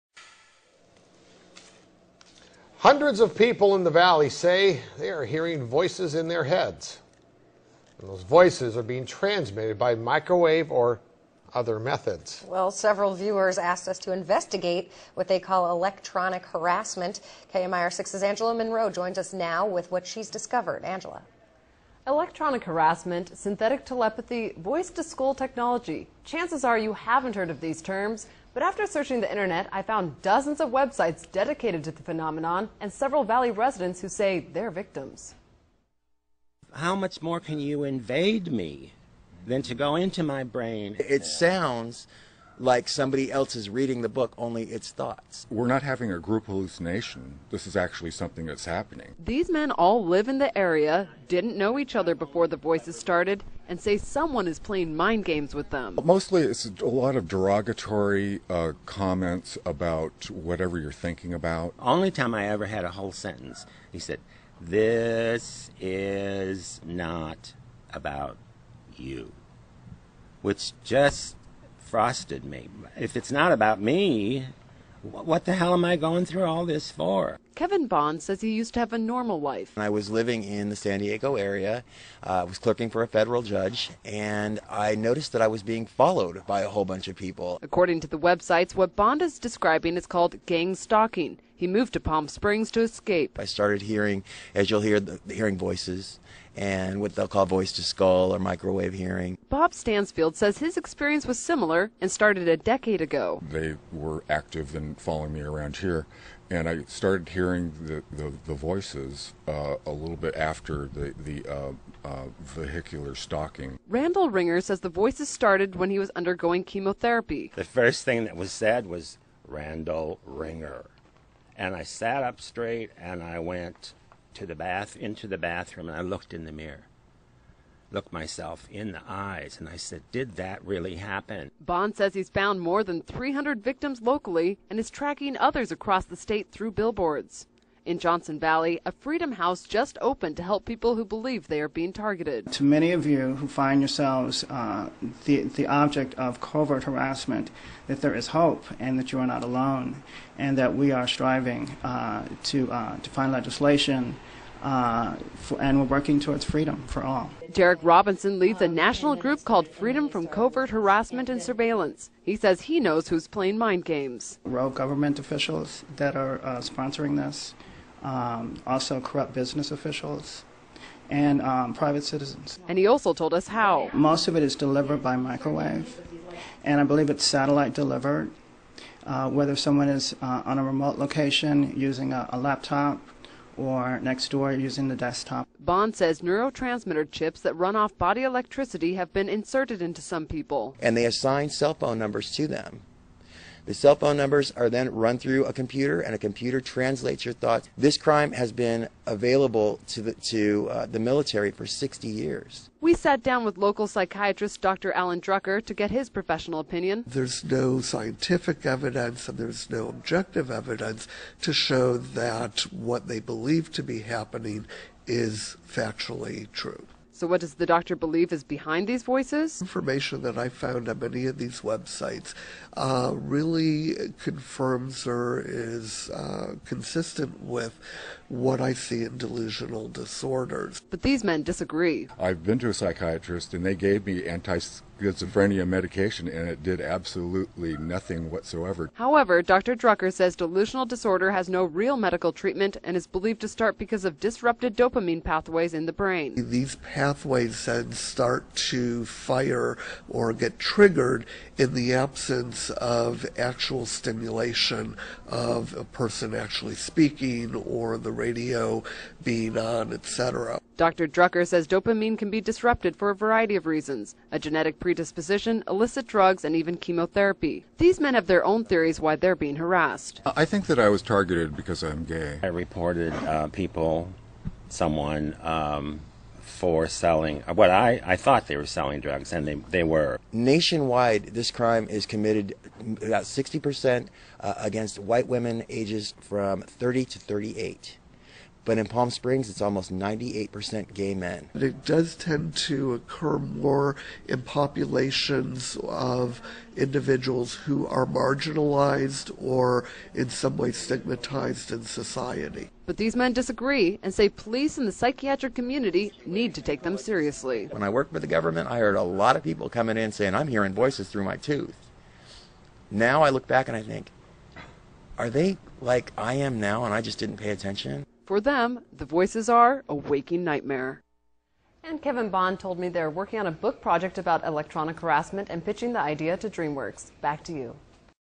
Microwave voices 中文 in skull technology - USA tv news
此电视节目制作组，应受害者要求，而制作了这个报道。
主流媒体，日益报道受害群体的申诉，对民众醒觉很重要。

Victims requested the tv program team to investigate and thus the production of this
news report.  More and more victims found they became targets of these covert
technology crime worldwide.